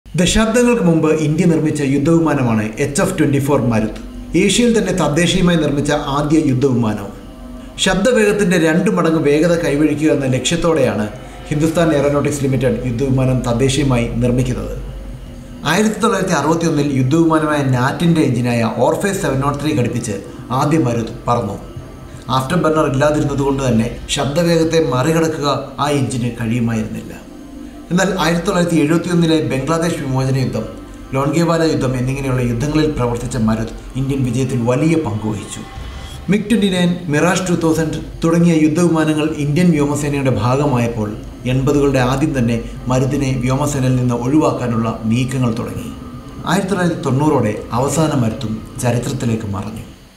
ഇന്ത്യ നിർമിച്ച ആദ്യത്തെ യുദ്ധ വിമാനം HF-24 മാരുത്
ഏഷ്യയിൽ തന്നെ തദ്ദേശീയമായി നിർമിച്ച ആദ്യ യുദ്ധവിമാനം. ശബ്ദ വേഗത്തിന്റെ രണ്ടുമടങ് വേഗത കൈവരിക്കുക എന്ന ലക്ഷ്യത്തോടെ ആണ് ഹിന്ദുസ്ഥാൻ എയ്റോനോട്ടിക്സ് ലിമിറ്റഡ്  യുദ്ധ വിമാനം തദ്ദേശീയമായി നിർമിക്കുന്നത്.

hindustan Aeronautics Limited